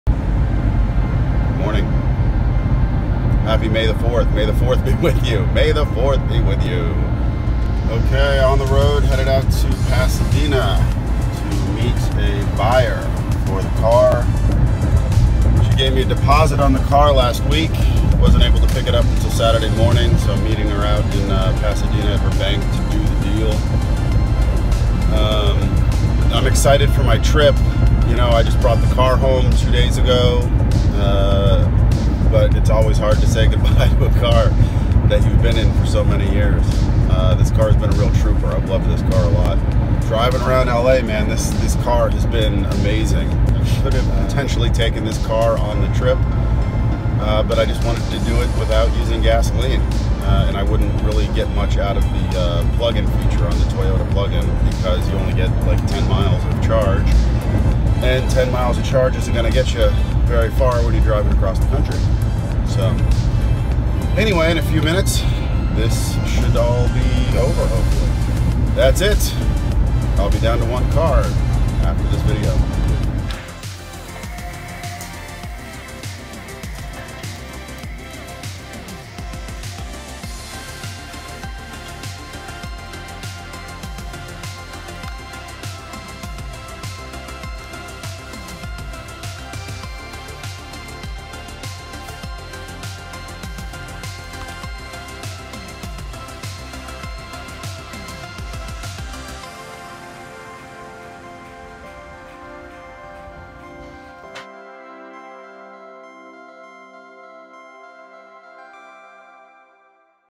Good-bye old friend... Saying bye to the old car to welcome the new Tesla Model 3
The Toyota Prius plug-in I've had for six years was sold just days after purchasing the new Tesla Model 3. The journey begins with a good-bye.